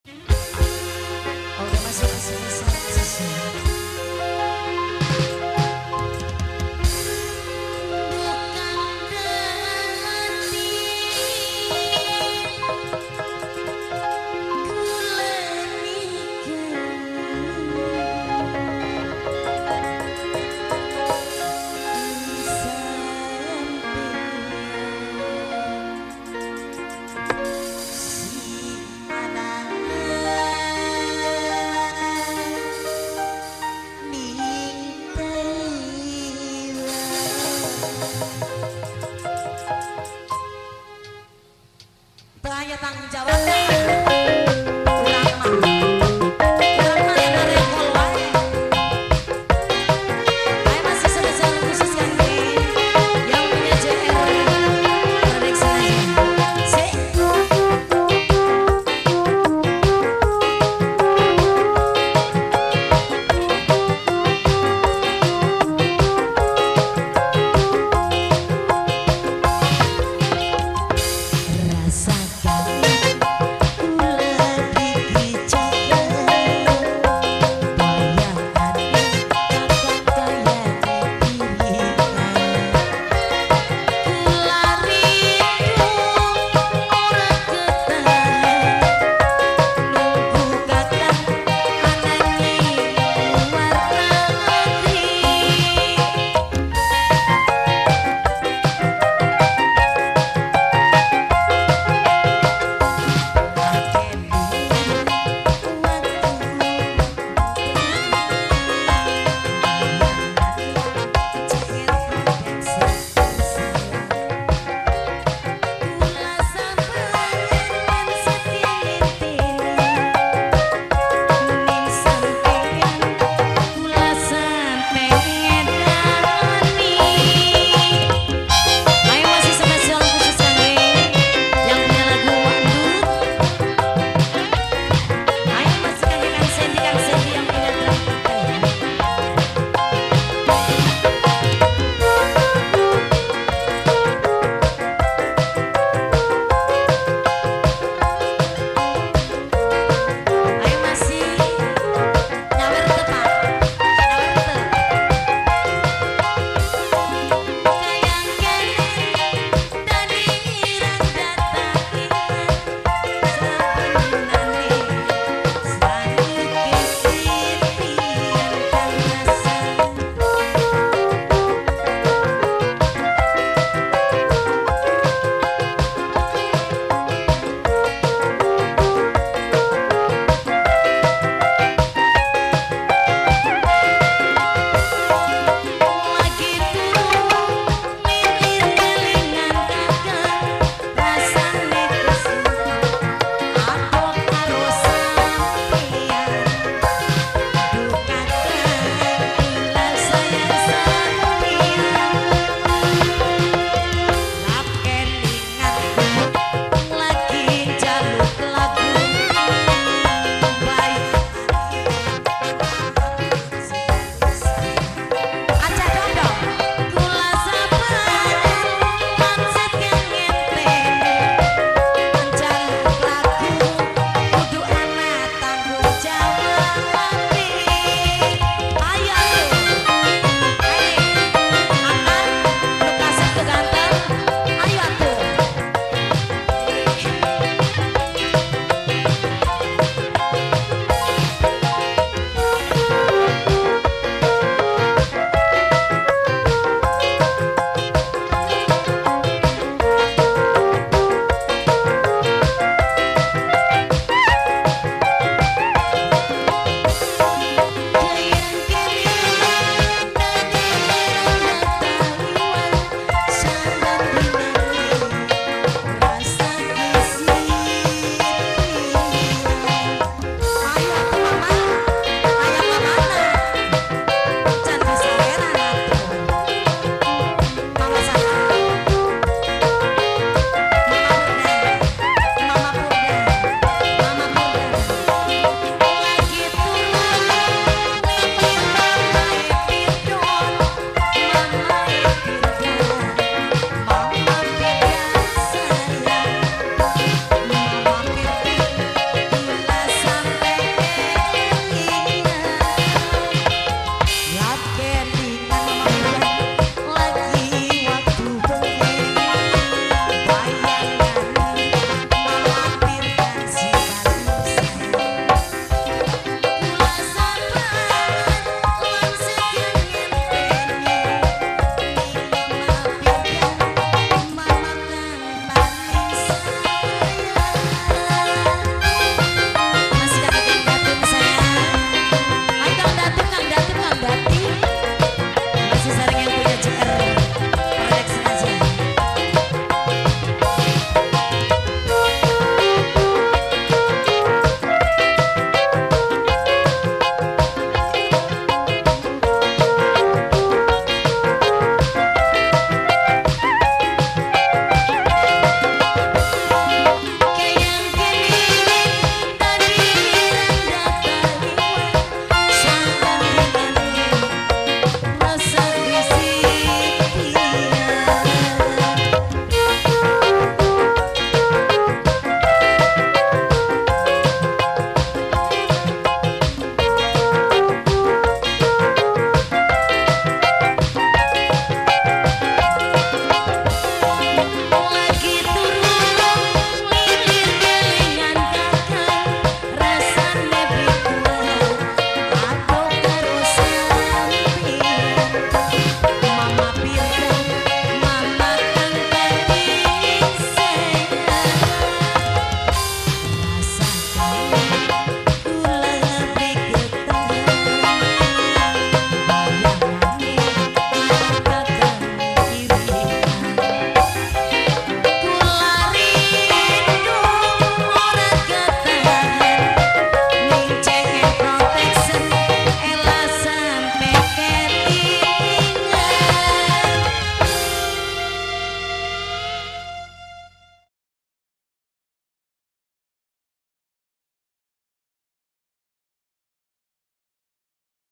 11 RANGDA TAIWAN # GAJAH PUTRA # PATOK BEUSI - SUBANG
Terimakasih sohib telah mengunjungi Channel Hiburan JR PRO\\. Dokumentasi Hajatnya : Kel. Bpk. Hanapi / Ibu Nasem. Khitanan : RAMDAN SIGIT. Edisi: Minggu 12 Oktober 2014 . Lokasi : Ds.Gempol Sari, Rt 11/04 Kec. Patok Beusi  Kab. Subang  ( Jabar)